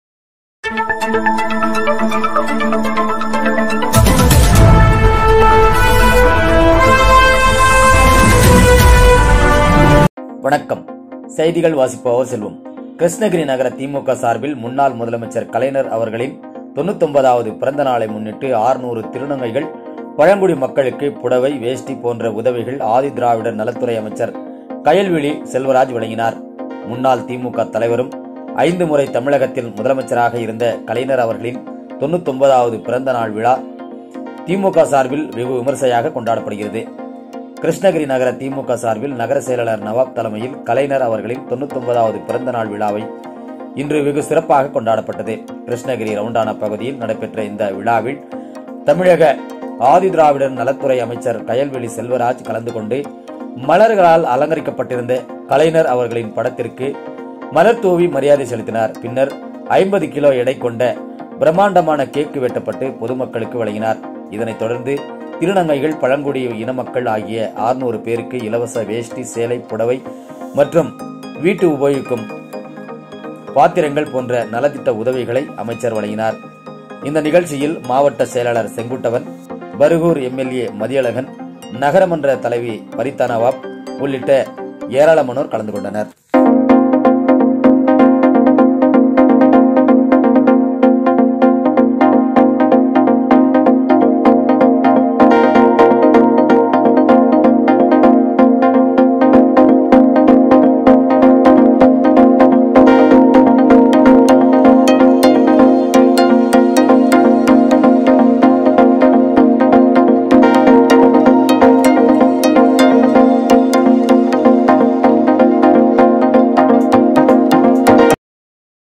கலைஞர் அவர்களின் 99 வது பிறந்த நாளை முன்னிட்டு 600 திருநங்கைகள்,பழங்குடி மக்களுக்கு புடவை,வேட்டி...
கிருஷ்ணகிரி நகர திமுக சார்பில் முன்னாள் முதலமைச்சர் கலைஞர் அவர்களின் 99 வது பிறந்த நாளை முன்னிட்டு 600 திருநங்கைகள், பழங்குடி மக்களுக்கு புடவை,வேட்டி, போன்ற உதவிகள் ஆதிதிராவிட நலத்துறை அமைச்சர் கயல்விழி செல்வராஜ் வழங்கினார்.
முன்னால் திமுக தலைவரும் ஐந்து முறை தமிழகத்தின் முதலமைச்சராக இருந்த கலைஞர் அவர்களின் 99 வது பிறந்த நாள் விழா திமுக சார்பில் வெகுவிமர்சையாக கொண்டாடப்படுகிறது.

கிருஷ்ணகிரி நகர திமுக சார்பில் நகர செயலாளர் நவாப் தலைமையில் கலைஞர் அவர்களின் 99 வது பிறந்தநாள் விழாவை இன்று வெகு சிறப்பாக கொண்டாடப்பட்டது. கிருஷ்ணகிரி ரவுண்டானா பகுதியில் நடைபெற்ற இந்த விழாவில் தமிழக ஆதிதிராவிடர் நலத்துறை அமைச்சர் கயல்விழி செல்வராஜ் கலந்து கொண்டு மலர்களால் அலங்கரிக்கப்பட்ட கலைஞர் அவர்களின் படத்திற்கு மலர் தூவி மரியாதை செலுத்தினார். பின்னர் 50 கிலோ எடை கொண்ட பிரமாண்ட கேக் வெட்டப்பட்டு பொதுமக்களுக்கு வழங்கினார். இதைத்தொடர்ந்து திருநங்கைகள் பழங்குடி இனமக்கள் ஆகிய 600 பேருக்கு இலவச வேஷ்டி புடவை மற்றும் வீட்டு உபயோக பாத்திரங்கள் போன்ற நலத்திட்ட உதவிகளை அமைச்சர் வழங்கினார். இந்த நிகழ்ச்சியில் மாவட்ட செயலாளர் செங்குட்டுவன் பர்கூர் எம்எல்ஏ மதியழகன் நகர மன்ற தலைவி பரிதாநவாப் உள்ளிட்ட ஏராளமானோர் கலந்து கொண்டனர்.